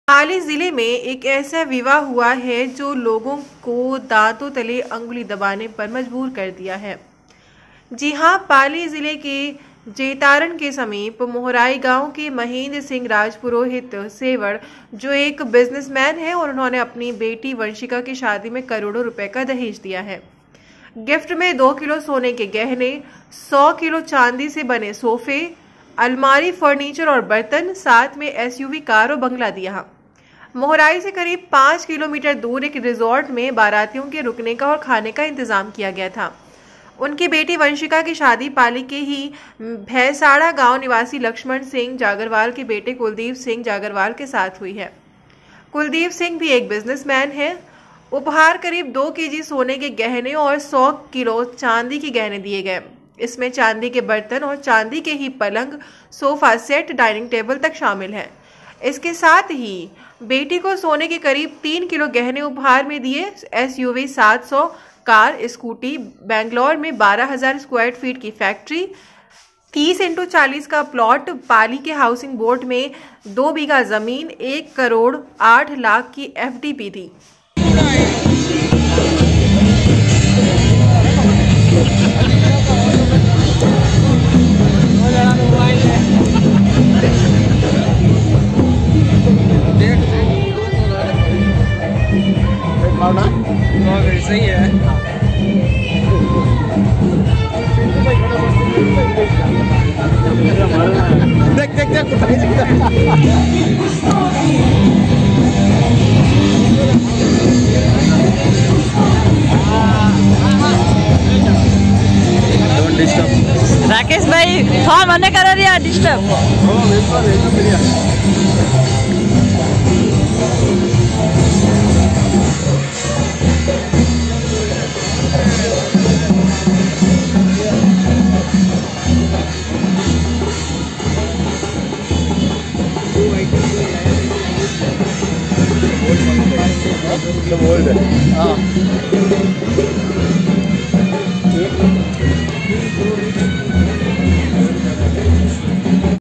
पिता ने अपनी बेटी की शादी में दिए करोड़ों रुपए का दहेज#gnewsindia
पिता ने अपनी बेटी की शादी में दिए  करोड़ों रुपए का दहेज,,,,
शादी को बनाया यादगार,,,,,

पाली,,

 पाली जिले में एक ऐसी  विवाह हुवा जो  को  लोगो को दातों तले अंगुली दबाने पर मजबूर कर दिया।  जी हां  पाली जिले के जेतारण के समीप  मोहराई गांव के मेंहद्र सिंह राजपुरोहित ( सेवड़ )  जो एक बिजनेस मैन हे और उन्होंने अपनी बेटी   वंसिका की शादी में  करोड़ों  रुपए का दहेज दिया।
  गिफ्ट में 2 किलो  सोने के गहने,  100 किलो  चांदी के से बने सोफे , अलमारी , फर्नीचर और बर्तन,  साथ में SUV कार और बंगला दिया। मोहराई से करीब 5 किमी दूर एक रिसॉर्ट में बारातियों के रुकने और खाने का इंतजाम किया गया था।
उनकी बेटी वंशिका की शादी पाली के ही भैसाणा गांव निवासी लक्ष्मण सिंह जागरवाल के बेटे कुलदीप सिंह जागरवाल के साथ हुई है। कुलदीप सिंह भी एक बिजनेसमैन हैं।
 उपहार  करीब 2 KG सोने के गहने और 100 KG चांदी के गहने दिए गए। इसमें चांदी के बर्तन और चांदी के ही पलंग, सोफा-सेट, डायनिंग टेबल तक शामिल हैं। इसके साथ ही सिर से लेकर पैरों तक पहनने के लिए बेटी को सोने के करीब 3 किलो गहने उपहार में दिए। SUV-700 कार, स्कूटी, बेंगलुरु में 12 हजार स्क्वायर फीट की फैक्ट्री, 30×40 का प्लॉट, पाली के हाउसिंग बोर्ड में 2 बीघा जमीन, एक करोड़ 8 लाख की FD दी।